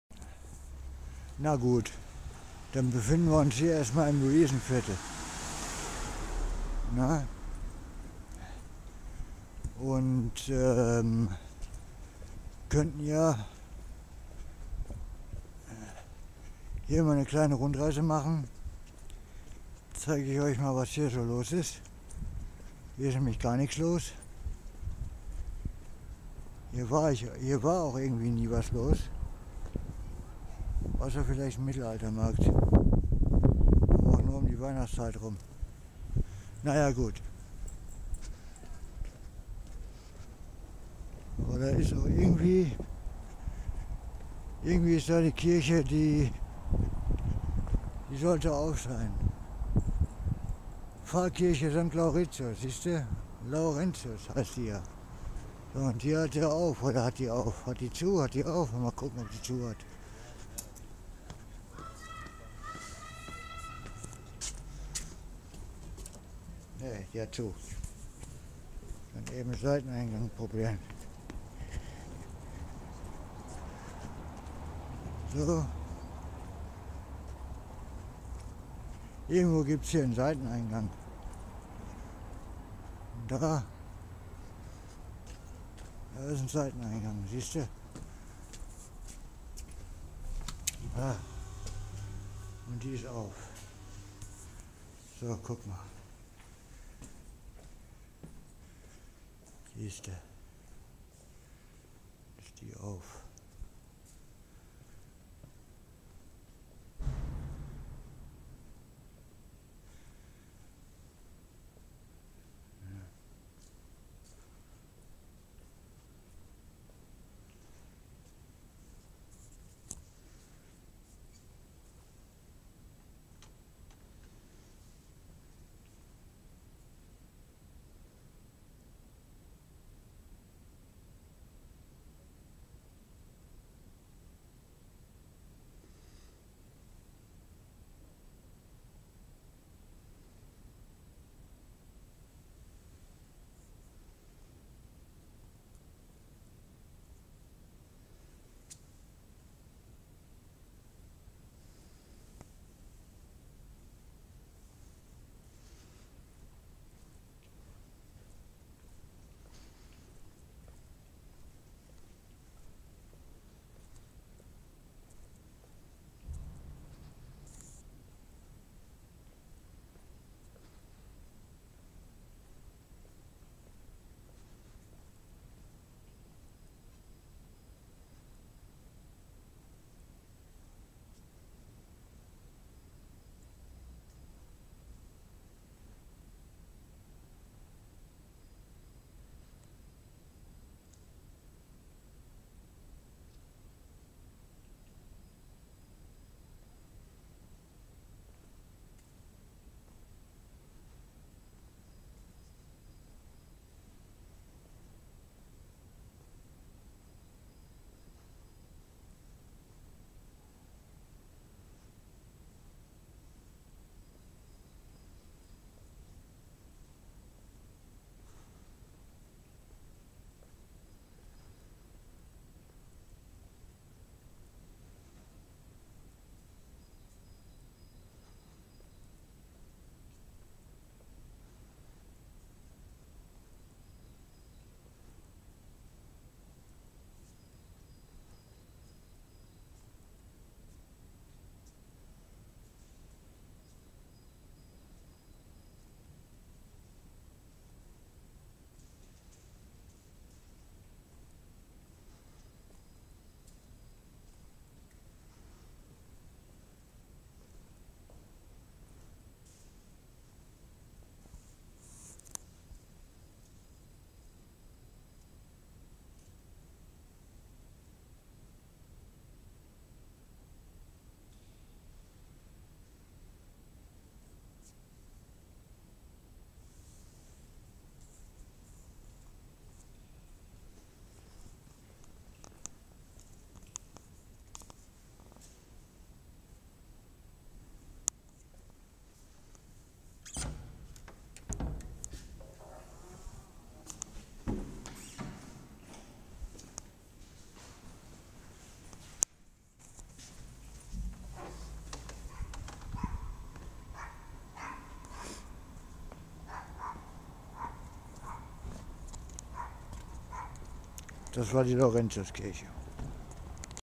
Luisenkirche
Dieses Video entstand bei einem Spaziergag.
durch die Kirche des Luisenvierels.